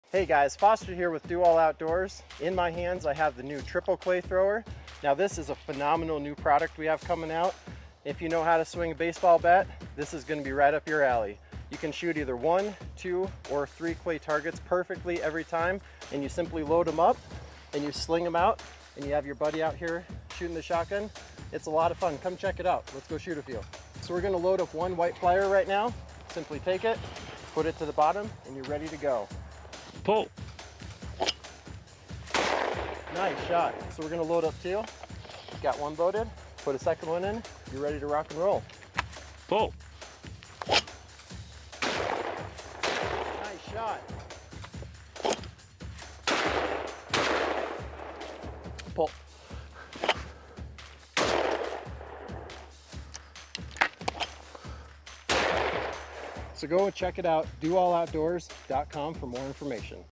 Triple Clay | BRAND NEW | The Homerun of Hand Throwers
Get ready to swing for the fences, you can now throw one, two or three clay pigeons at once with ease!